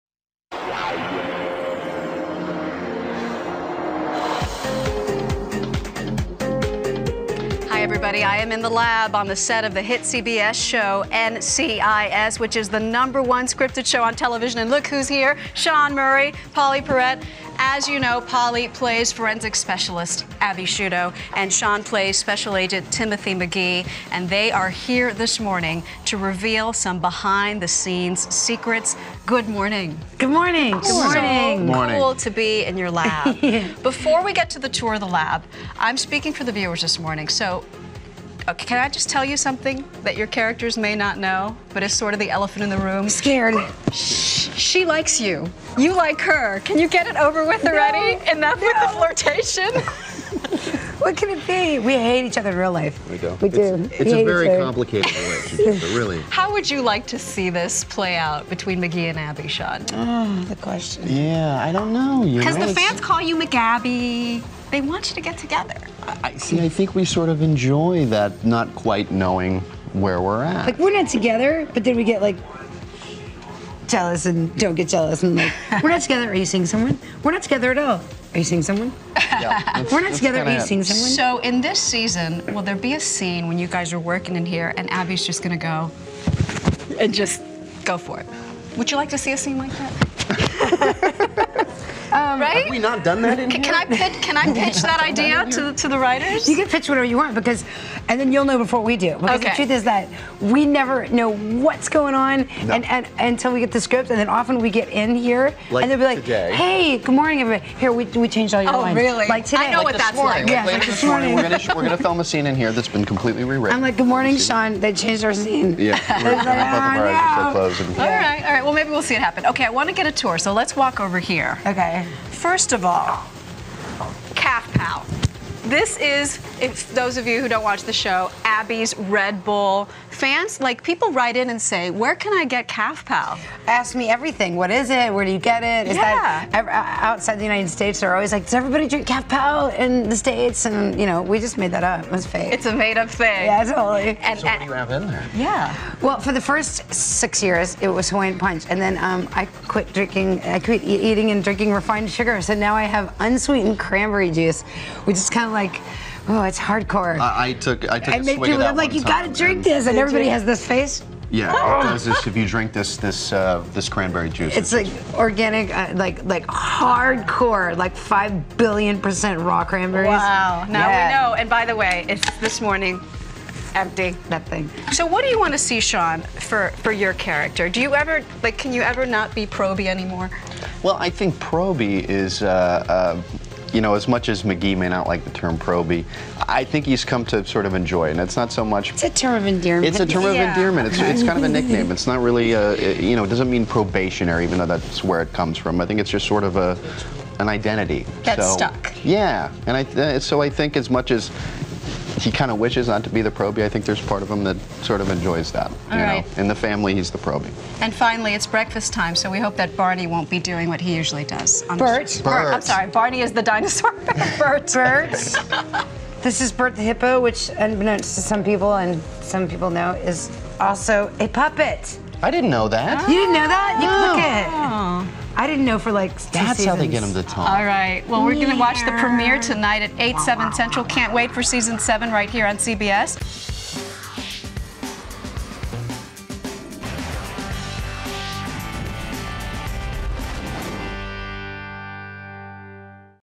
NCIS Set Secrets
Maggie Rodriguez spoke with NCIS cast members Pauley Perrette and Sean Murray about the season premiere on the set of the hit CBS show.